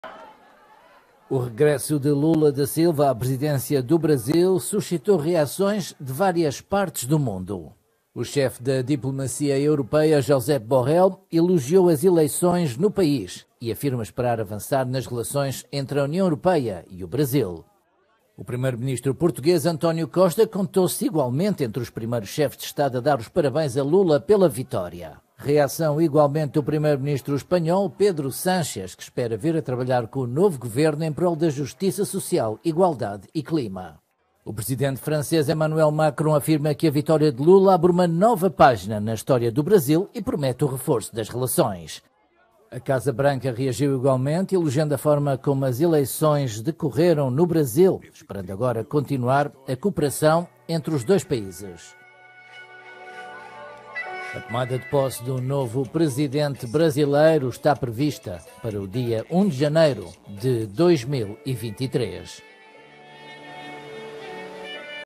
Europa e EUA reagem à vitória de Lula no Brasil
A União Europeia e os Estados Unidos prometem reforço as relações com o Brasil